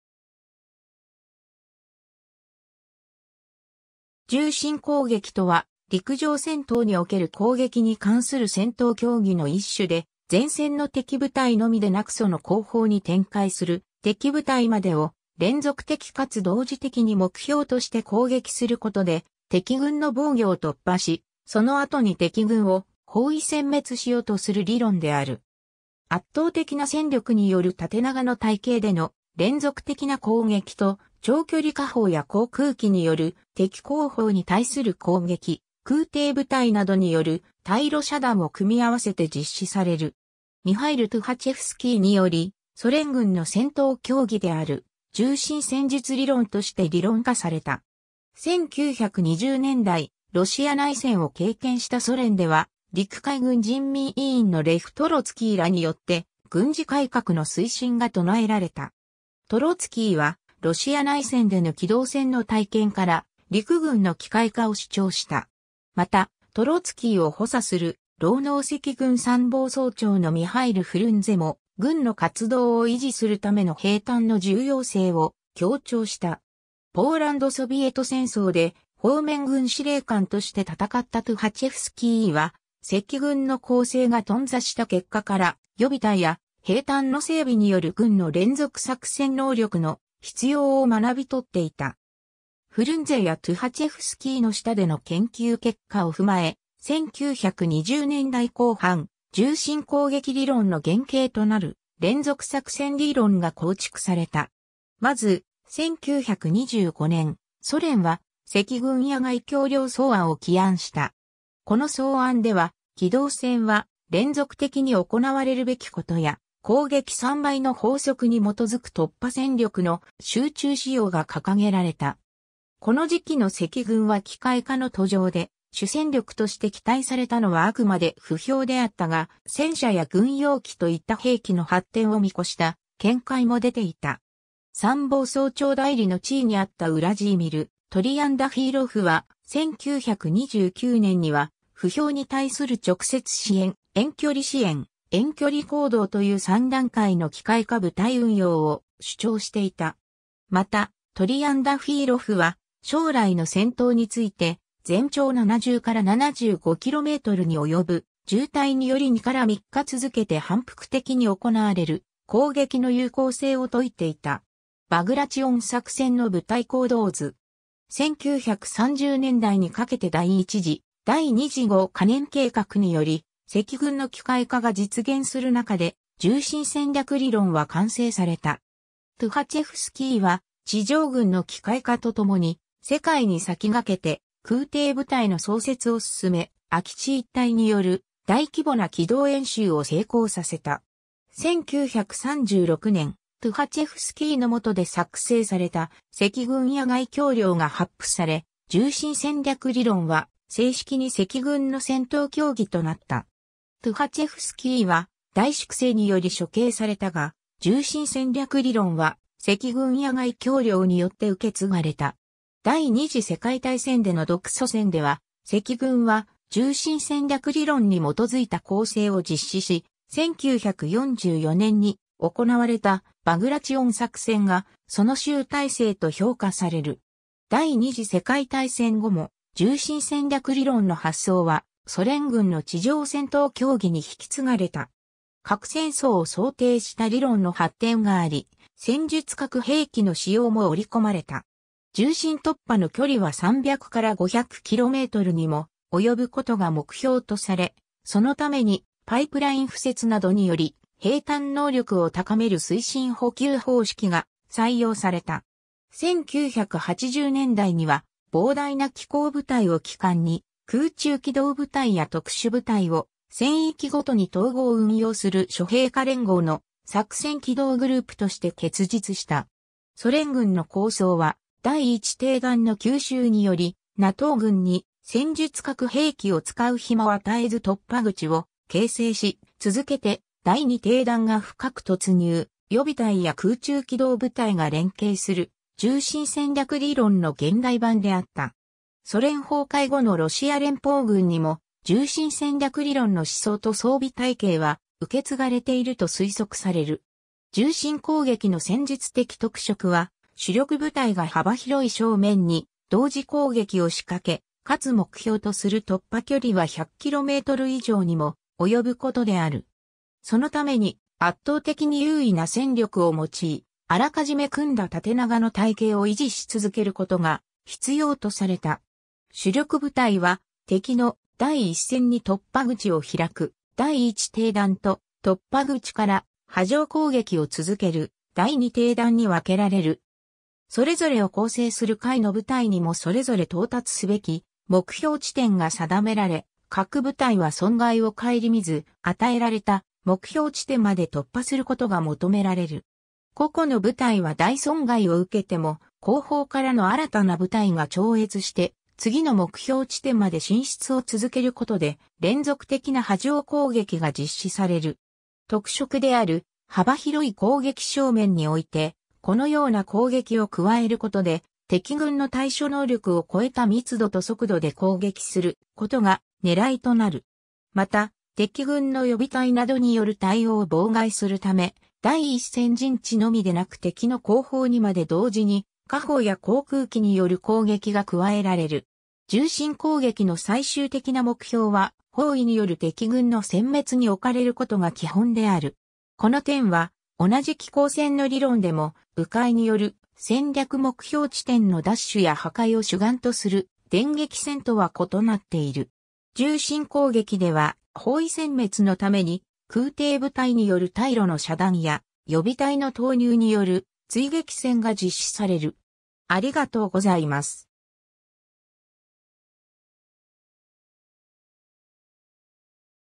縦深攻撃
縦深攻撃（じゅうしんこうげき、英語: Deep battle）とは、陸上戦闘における攻撃に関する戦闘教義の一種で、前線の敵部隊のみでなくその後方に展開する敵部隊までを連続的かつ同時的に目標として攻撃することで敵軍の防御を突破し、その後に敵軍を包囲殲滅しようとする理論である。
圧倒的な戦力による縦長の隊形での連続的な攻撃と、長距離火砲や航空機による敵後方に対する攻撃、空挺部隊などによる退路遮断を組み合わせて実施される。
ミハイル・トゥハチェフスキーにより、ソ連軍の戦闘教義である縦深戦術理論（縦深戦略理論とも）として理論化された。
 1920年代、ロシア内戦を経験したソ連では、陸海軍人民委員（国防相に相当）のレフ・トロツキーらによって軍事改革の推進が唱えられた。
トロツキーは、ロシア内戦での機動戦の体験から、陸軍の機械化を主張した。
また、トロツキーを補佐する労農赤軍参謀総長のミハイル・フルンゼも、軍の活動を維持するための兵站の重要性を強調した。
ポーランド・ソビエト戦争で方面軍司令官として戦ったトゥハチェフスキーは、赤軍の攻勢が頓挫した結果から、予備隊や兵站の整備による軍の連続作戦能力の必要を学びとっていた。
 フルンゼやトゥハチェフスキーの下での研究結果をふまえ、1920年代後半、縦深攻撃理論の原型となる連続作戦理論が構築された。
まず、1925年、ソ連は『赤軍野外教令草案』を起案した。
この草案では、機動戦は連続的に行われるべきことや、攻撃三倍の法則に基づく突破戦力の集中使用が掲げられた。
この時期の赤軍は機械化の途上で主戦力として期待されたのはあくまで歩兵であったが、戦車や軍用機と言った兵器の発展を見越した見解も出ていた。
参謀総長代理の地位にあったウラジーミル・トリアンダフィーロフは、1929年には、歩兵に対する直接支援、遠距離支援、遠距離行動という三段階の機械化部隊運用を主張していた。
また、トリアンダフィーロフは、将来の戦闘について、全長70-75kmに及ぶ縦隊により2-3日続けて反復的に行われる攻撃の有効性を説いていた。
 バグラチオン作戦の部隊行動図。
 1930年代にかけて第一次/第二次五カ年計画により赤軍の機械化が実現する中で、縦深戦略理論は完成された。
トゥハチェフスキーは、地上軍の機械化とともに、世界に先駆けて空挺部隊の創設を進め、空地一体による大規模な機動演習を成功させた。
1936年、トゥハチェフスキーの下で作成された『赤軍野外教令』が発布され、縦深戦略理論は正式に赤軍の戦闘教義となった。
トゥハチェフスキーは大粛清により処刑されたが、縦深戦略理論は『赤軍野外教令』によって受け継がれた。
第二次世界大戦での独ソ戦では、赤軍は、縦深戦略理論に基づいた攻勢を実施し、1944年に行われたバグラチオン作戦がその集大成と評価される。
 第二次世界大戦後も、縦深戦略理論の発想はソ連軍の地上戦闘教義に引き継がれた。
核戦争を想定した理論の発展があり、戦術核兵器の使用も織り込まれた。
縦深突破の距離は300-500㎞にも及ぶことが目標とされ、そのためにパイプライン敷設などにより兵站能力を高める推進補給方式が採用された。
1980年代には、膨大な機甲部隊を基幹に空中機動部隊や特殊部隊を戦域ごとに統合運用する諸兵科連合の作戦機動グループ（Operational Maneuver Group, OMG）として結実した。
ソ連軍の構想は、第一梯団の急襲によりNATO軍に戦術核兵器を使う暇を与えず突破口を形成し、続けて第二梯団が深く突入、予備隊や空中機動部隊が連携する縦深戦略理論の現代版であった。
 ソ連崩壊後のロシア連邦軍にも、縦深戦略理論の思想と装備体系は受け継がれていると推測される。
 縦深攻撃の戦術的特色は、主力部隊が幅広い正面に同時攻撃を仕掛け、かつ目標とする突破距離（縦深）は100km以上にも及ぶことである。
そのために、圧倒的に優位な戦力を用い、あらかじめ組んだ縦長の隊形を維持し続けることが必要とされた。
 主力部隊は、敵の第一線に突破口を開く第一梯団と、突破口から波状攻撃を続ける第二梯団に分けられる。
それぞれを構成する下位の部隊にもそれぞれ到達すべき目標地点が定められ、各部隊は損害を顧みず与えられた目標地点まで突破することが求められる。
個々の部隊は大損害を受けても、後方からの新たな部隊が超越して次の目標地点まで進出を続けることで、連続的な波状攻撃が実施される。
特色である幅広い攻撃正面におい...